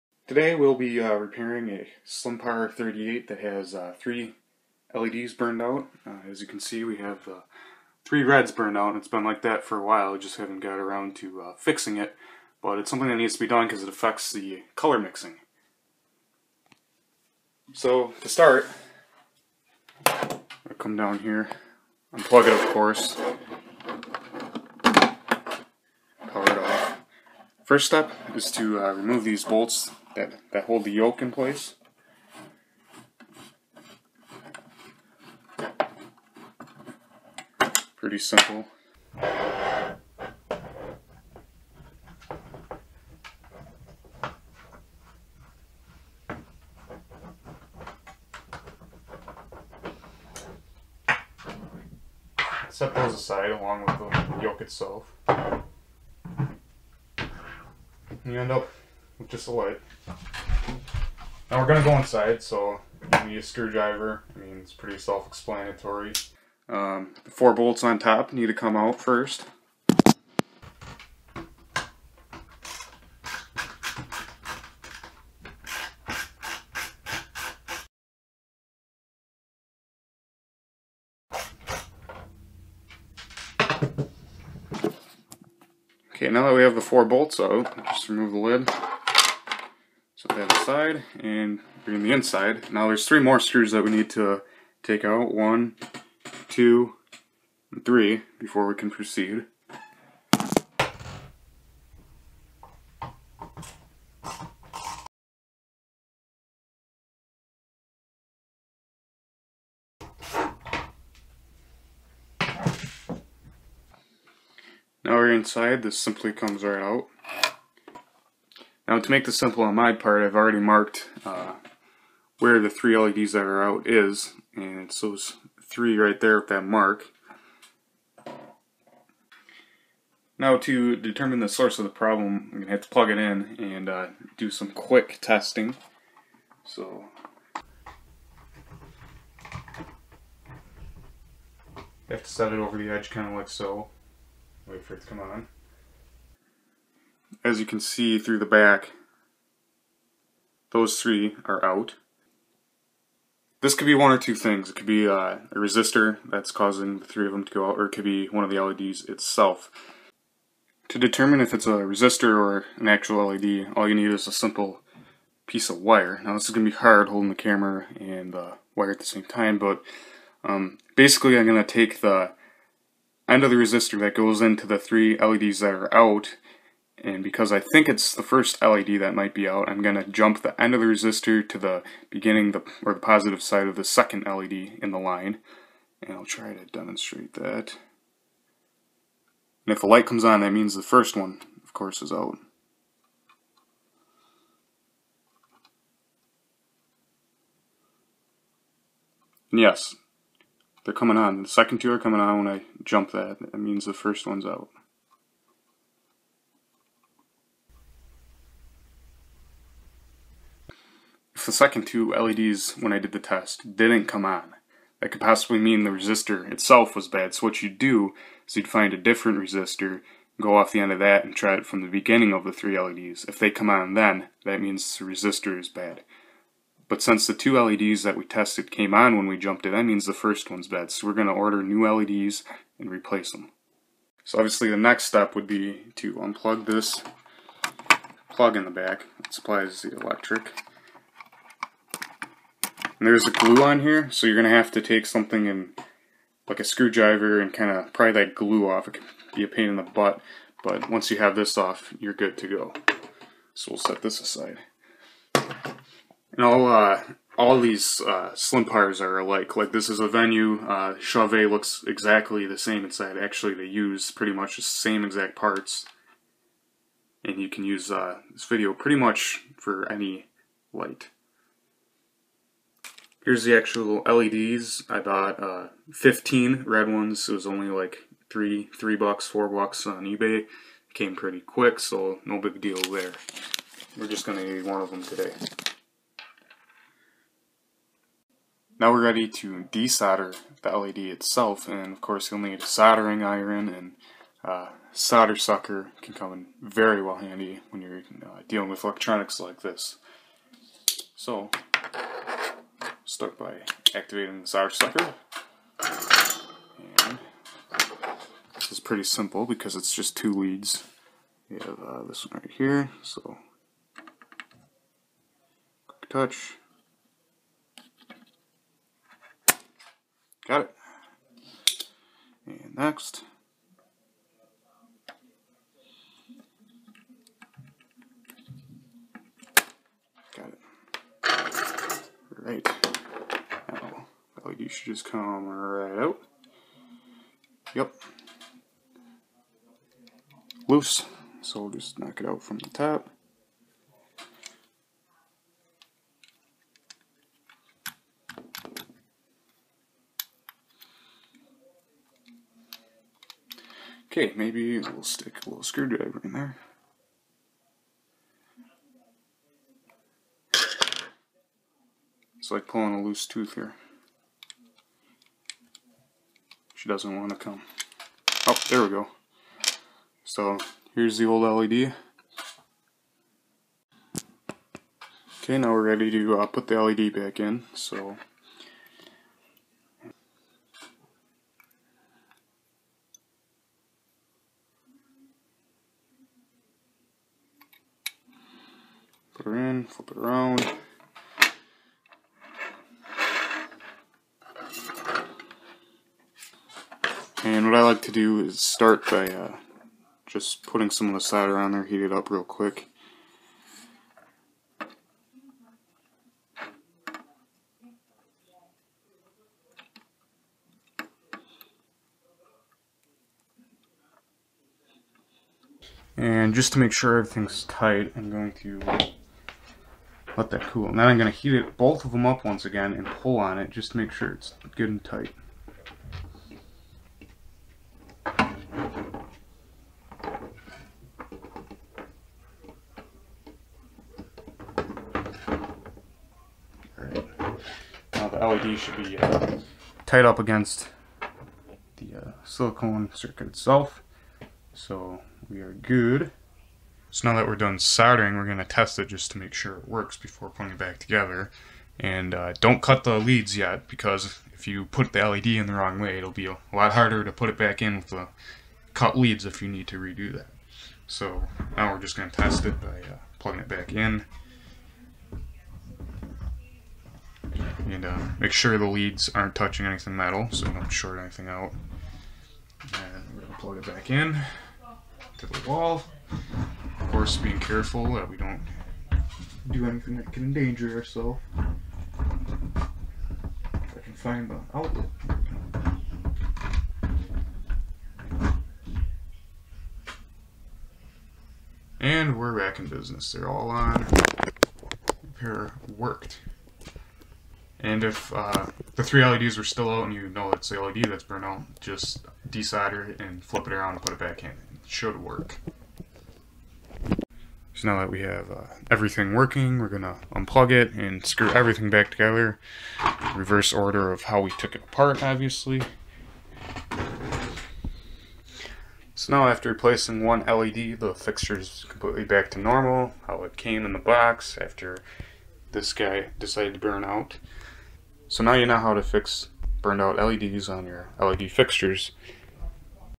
Led Repair Slimpar Dmx Light
3 leds were out and the process to diagnose and repair the light are explained.
Fixture is a Venue Thinpar 38: Guitar Center's comparable line to Chauvet.
This process can be use on nearly all led fixtures
Note: Performing this kind of work voids the manufacture warranty
If your a DIY this is the project for you